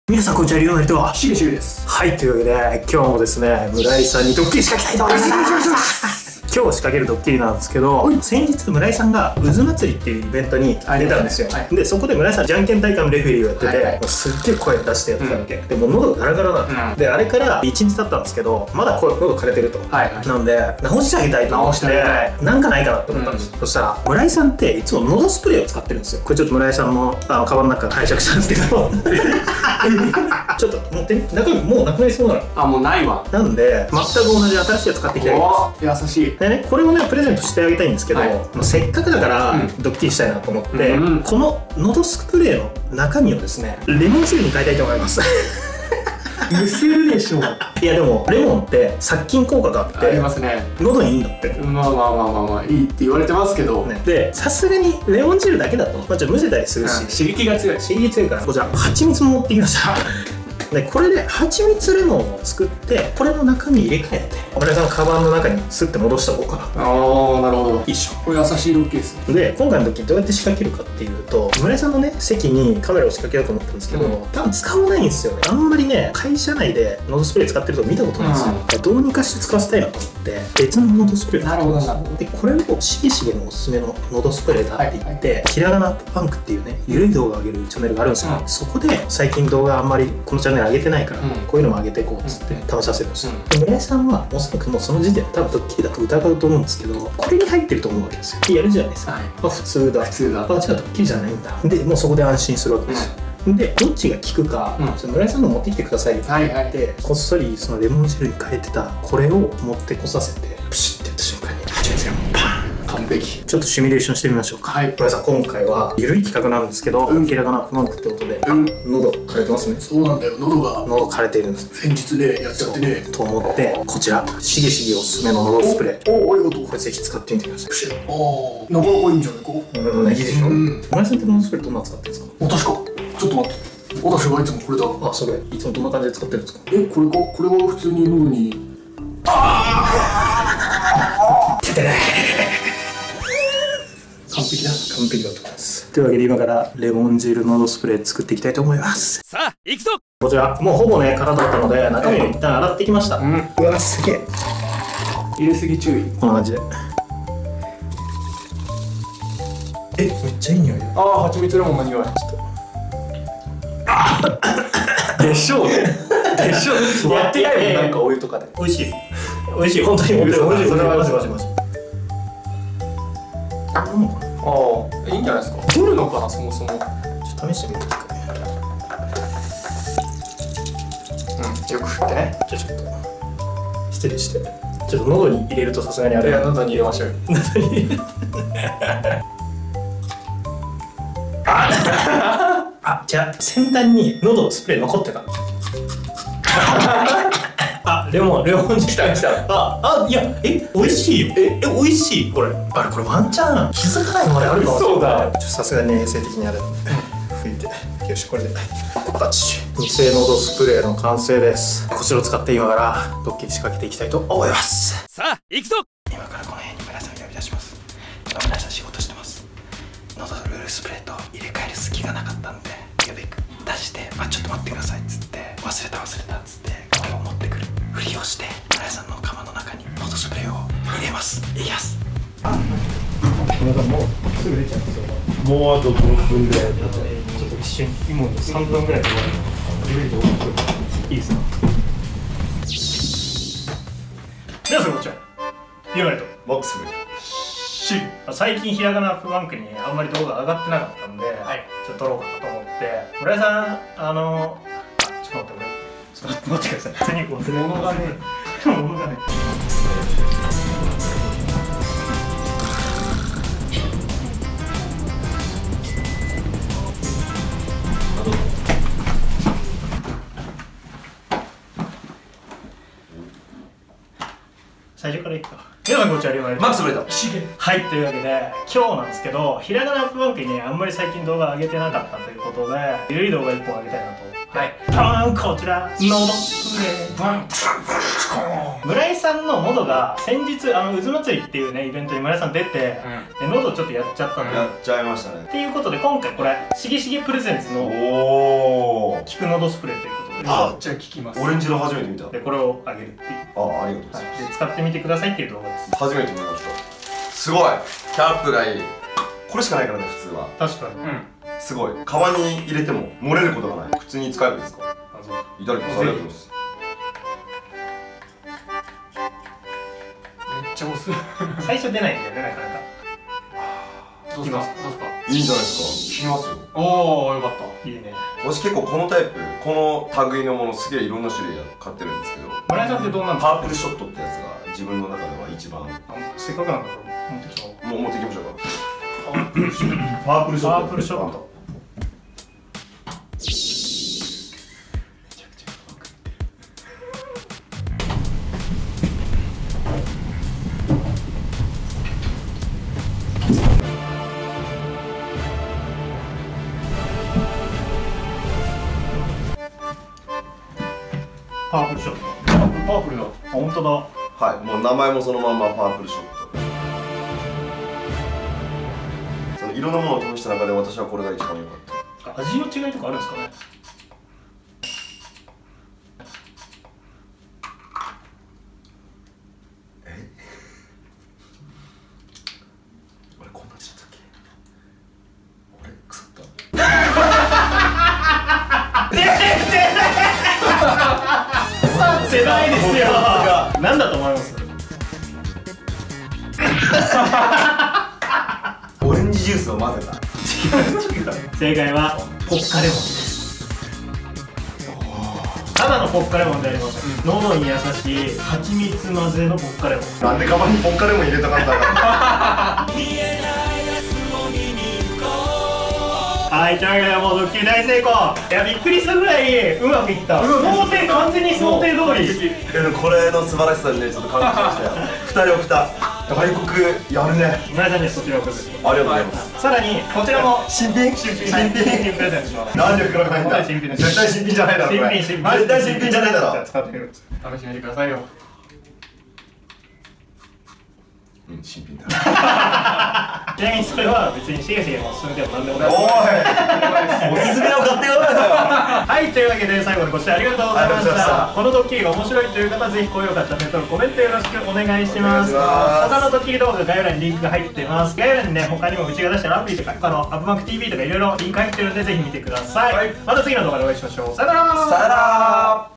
【ドッキリ】のどスプレーの中身をレモン汁に入れ替えた結果ｗｗｗ
★ドッキリリクエスト受付中！

★GACKTさん、マックスむらいも遊んでる！
ポーカーアプリ 大好評配信中！

★ニコニコで週２〜４回 生放送やってます！